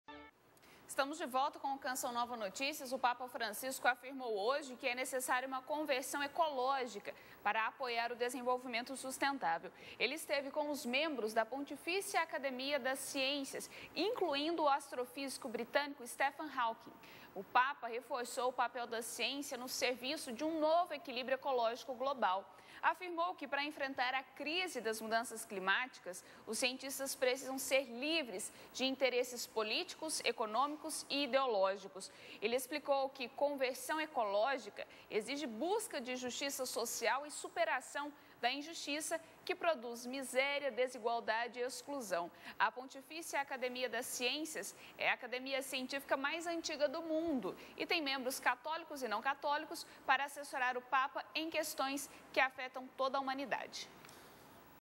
Papa afirma que é necessário haver conversão ecológica - CN Notícias
O Papa Francisco afirmou hoje que é necessária uma conversão ecológica para apoiar o desenvolvimento sustentável. Ele esteve com os membros da Pontifícia Academia das Ciências, incluindo o astrofísico britânico Stephen Hawking. O Papa reforçou o papel da ciência no serviço de um novo equilíbrio ecológico global. Afirmou que para enfrentar a crise das mudanças climáticas, os cientistas precisam ser livres de interesses políticos, econômicos e ideológicos. Ele explicou que conversão ecológica exige busca de justiça social e superação da injustiça que produz miséria, desigualdade e exclusão. A Pontifícia Academia das Ciências é a Academia científica mais antiga do mundo e tem membros católicos e não católicos para assessorar o Papa em questões que afetam toda a humanidade.